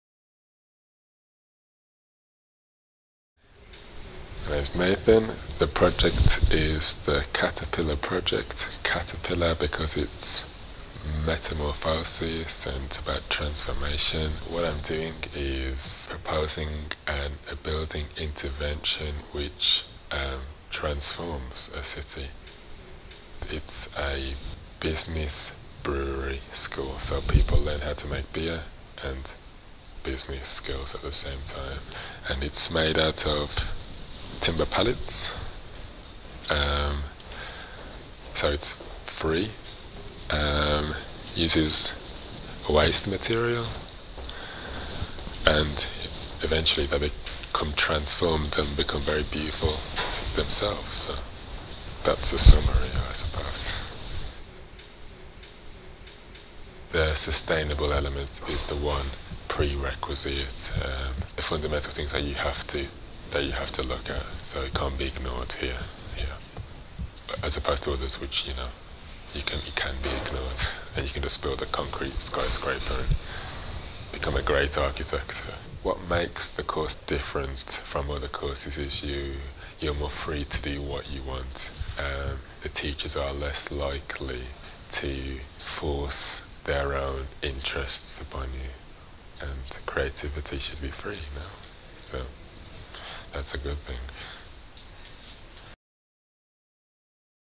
Graduate school Architecture Exhibition at the Centre for Alternative Technology
Professional Diploma in Architecture: Advanced Environmental and Energy Studies.  sustainable development
Students from the Graduate School of Environment at the Centre for Alternative Technology take us through their finals exhibition.

Nathan